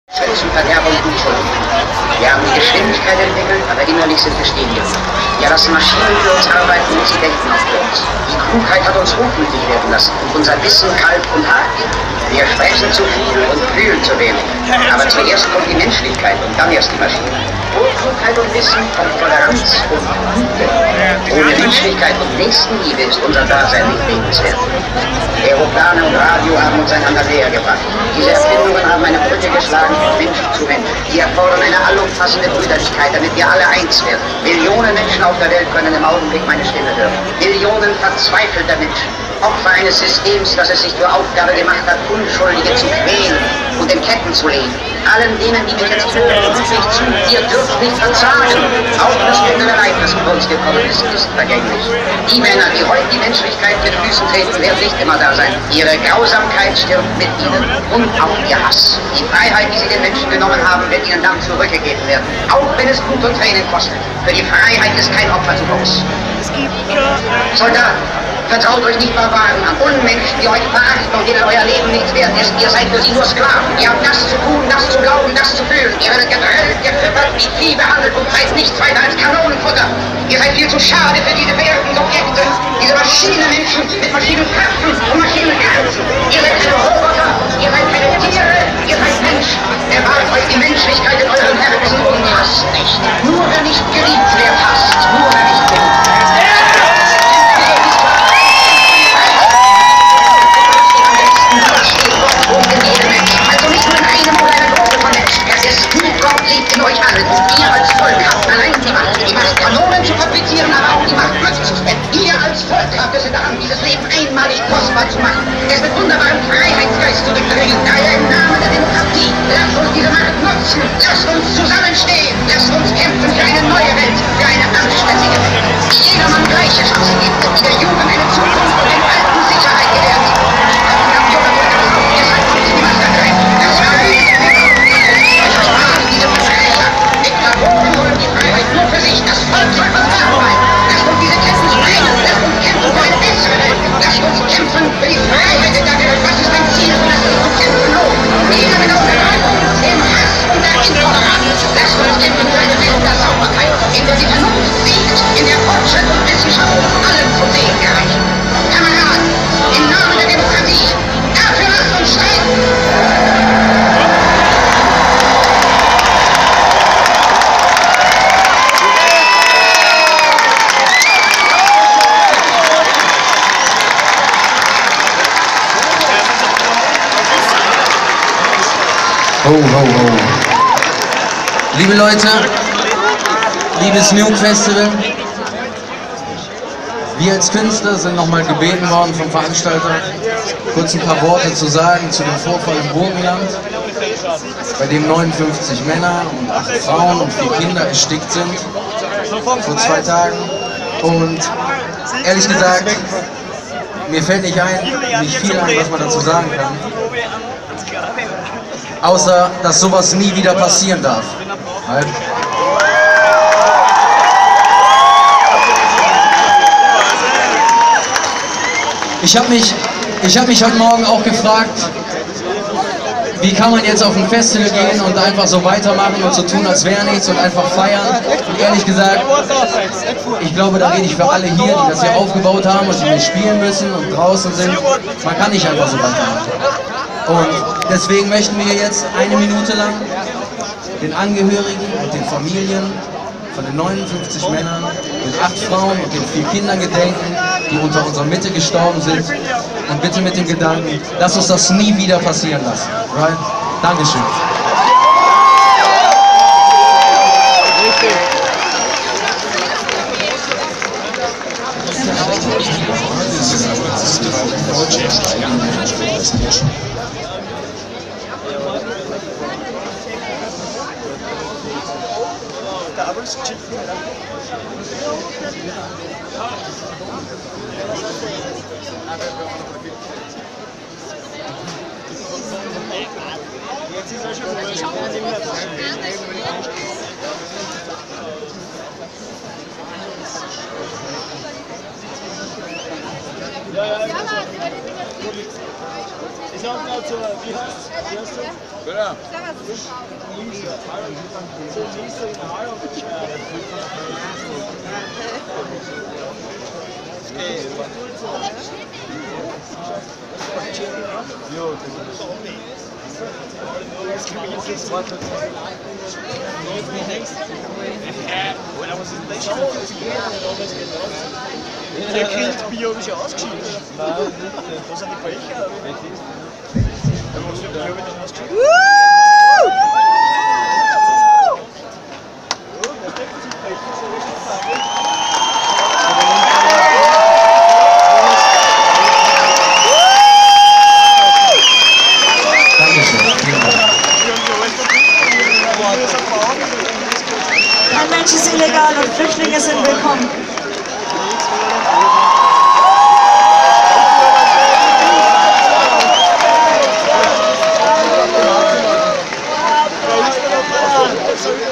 Nuke Festival 2015 - Refugees Welcome Ansprache
Ansprache und Schweigeminute für die Flüchtlinge die auf der Ladefläche eines LKW gestorben sind.